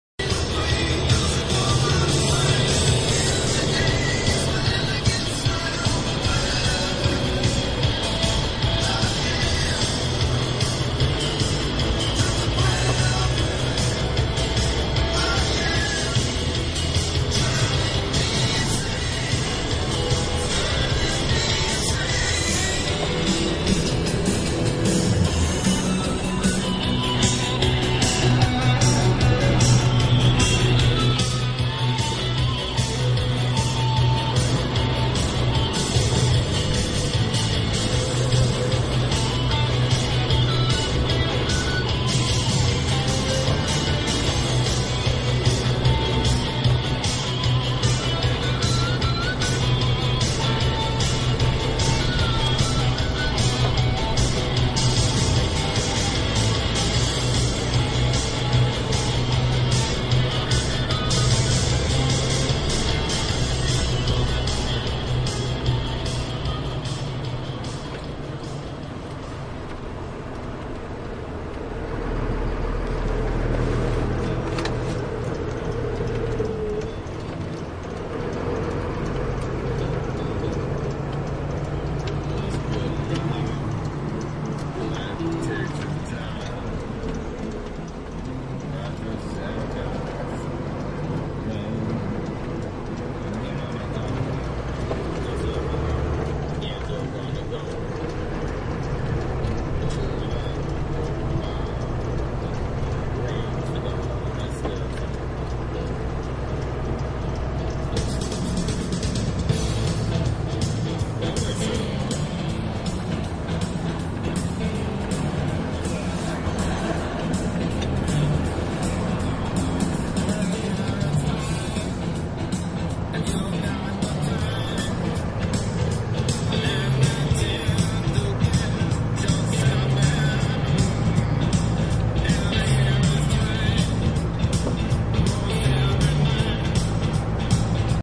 Vestfold vinter.wmv
Volvo FH12 520 hauling i light load through vestfold in Norway